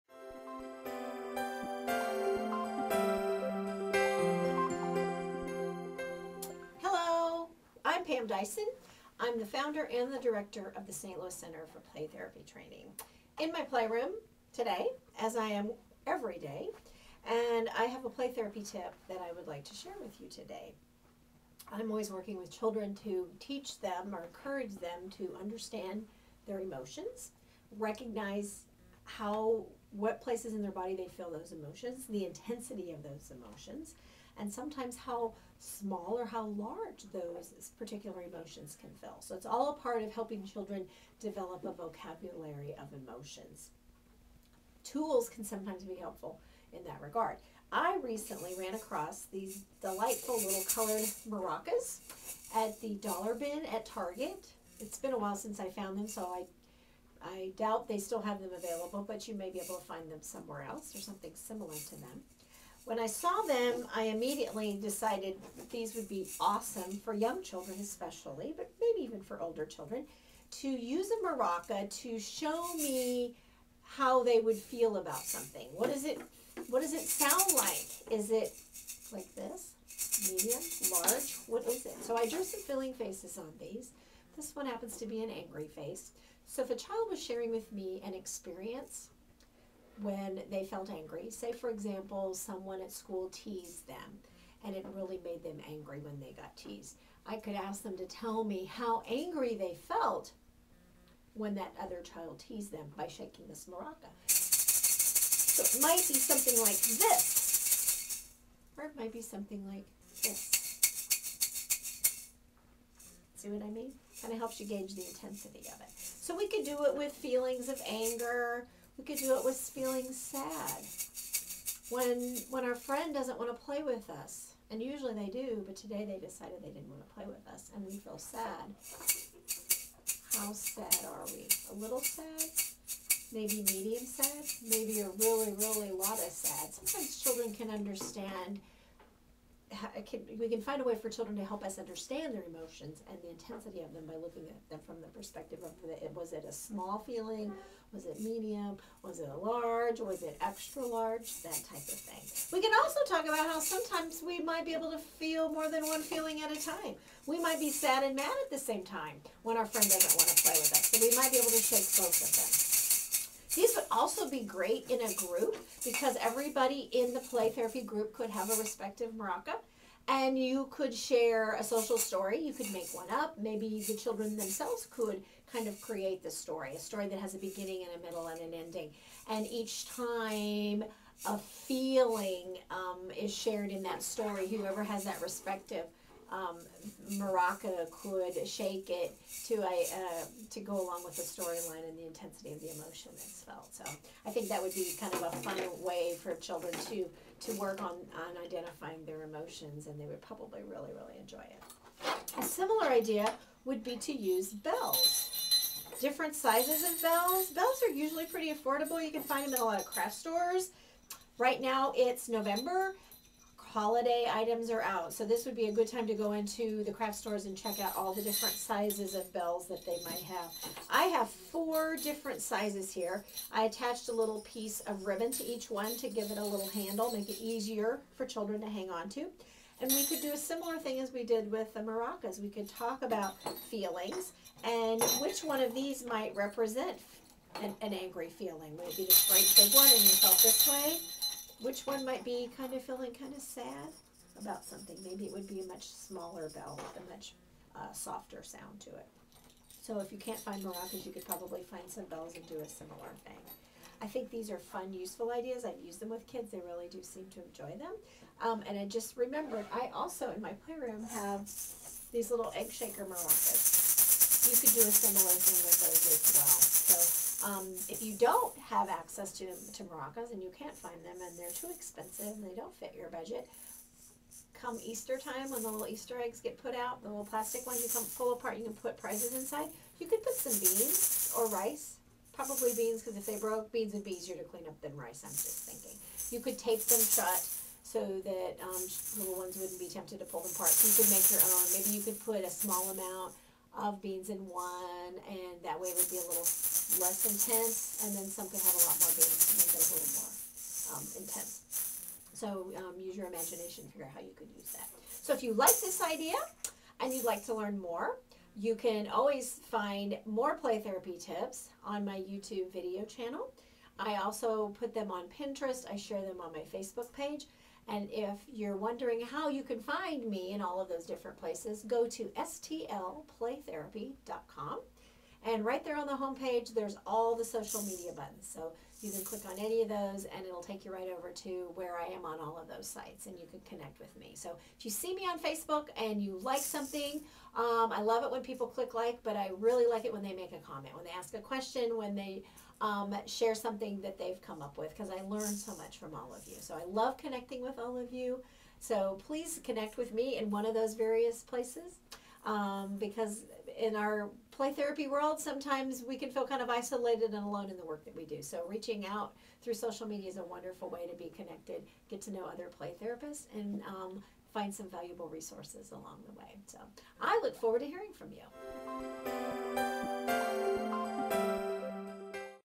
What Do Feelings Sound Like?
As a play therapist I use toys and other materials to help children find ways of expressing emotions and the intensity of those emotions. You can use maracas and bells as tools to encourage children to share what their feelings sound like.